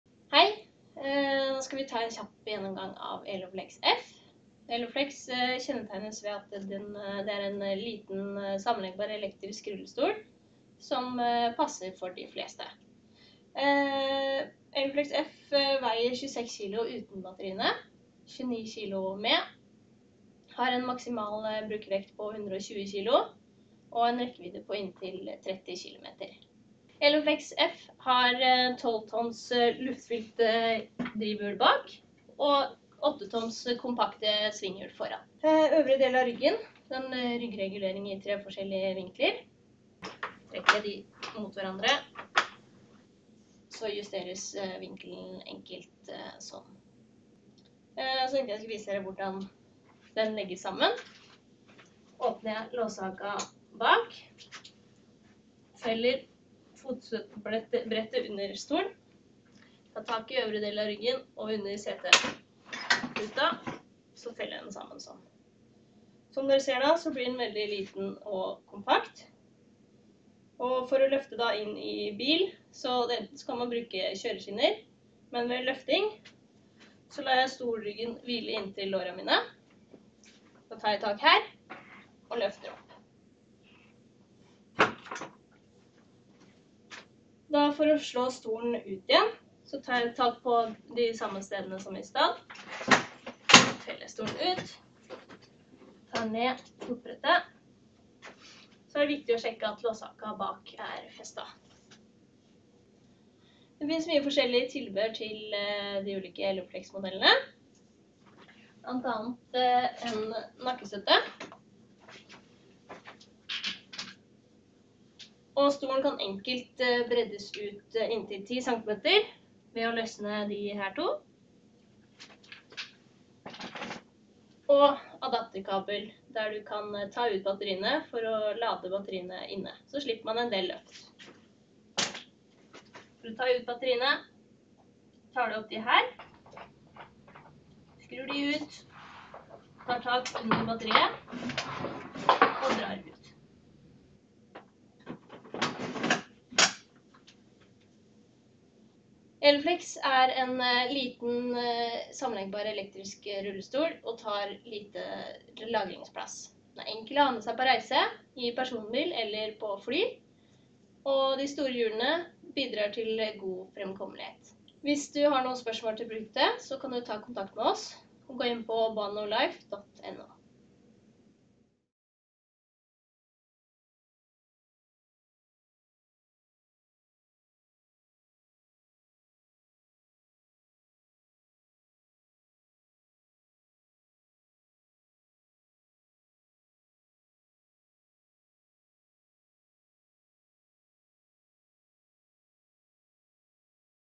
Eloflex F produktdemonstrasjon
Kort demonstrasjon av den sammenleggbare elektriske rullestolen Eloflex F.

Eloflex F er på rameavtale med NAV i posten/kategorien for elektrisk rullestol med motorisert styring for begrenset utendørs bruk, delbar eller sammenleggbar, for voksne. Delkontrakt 12.

Eloflex F er egnet for mennesker i alle aldre, behov eller bruk. Den sammenleggbare elektriske rullestolen er utstyrt med avansert teknologi som børsteløse kraftige motorer og doble litiumbatterier som standard og har en overlegen kjørekomfort med en rekkevidde på 30 km. Den kan enkelt tas med i bil da den er såpass liten og kompakt. Batteriene er flygodkjente, hvilket betyr at du også kan ta den med ombord på flyet da du skal ut å reise.

De store, luftfylte bakhjulene gir god fremkommelighet på ulike underlag. Svingradiusen er liten og presisjonen høy, hvilket innebærer at den fungerer like bra innendørs som utendørs. Med en Eloflex kan du enkelt navigere deg rundt på trange plasser og gjennom smale dører. Stolen kan enkelt tilpasses ved at den har tre reguleringsmuligheter i øvre del av ryggen og kan justeres til tre ulike setebredder.